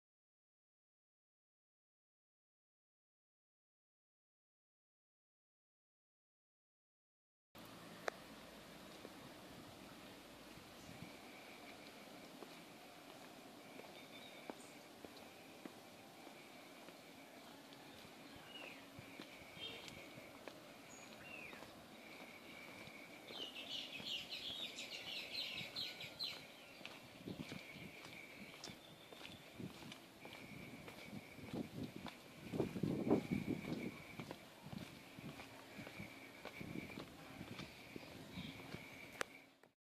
Andador "El Olmo"
Guarnición, pavimentación  y barandal en andador "El Olmo"